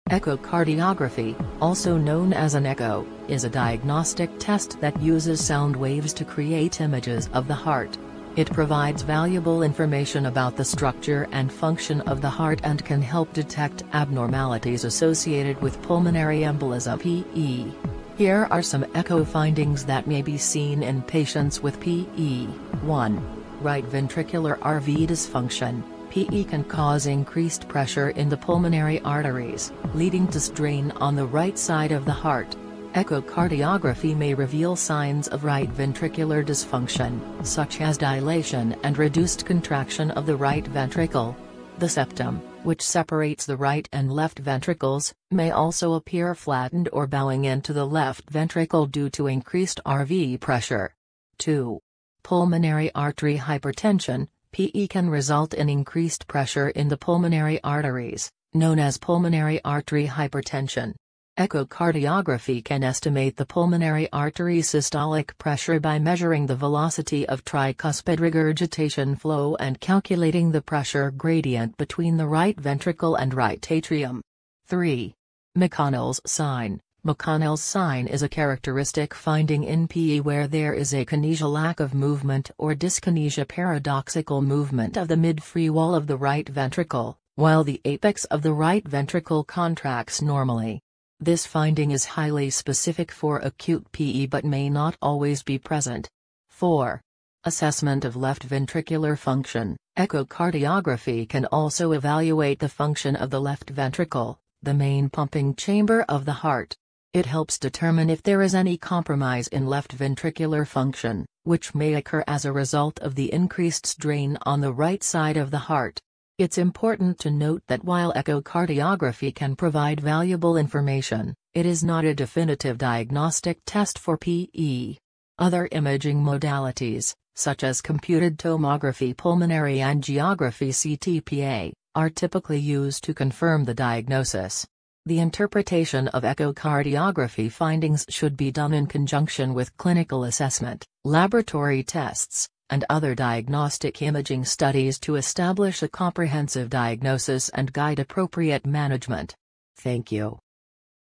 ECHO FINDINGS IN PULMONARY EMBOLISM
In this video, we provide a concise yet comprehensive overview of echo findings in pulmonary embolism (PE). Pulmonary embolism is a serious condition characterized by the blockage of arteries in the lungs by blood clots. Echocardiography, a non-invasive imaging technique, plays a crucial role in evaluating the impact of PE on the heart.